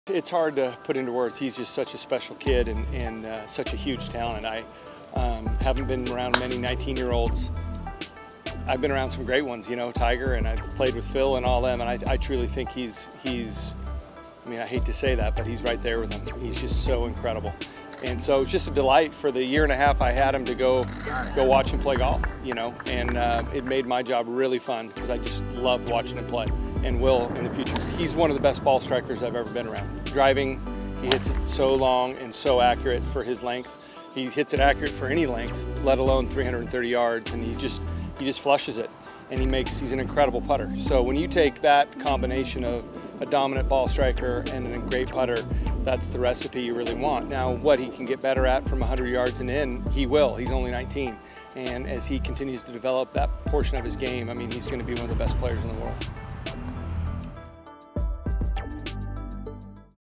Oregon's Casey Martin on Norman Xiong: 'He's going to be one of the best players in the world'
Oregon's head coach Casey Martin gives Norman Xiong high praise "He's going to be one of the best players in the world."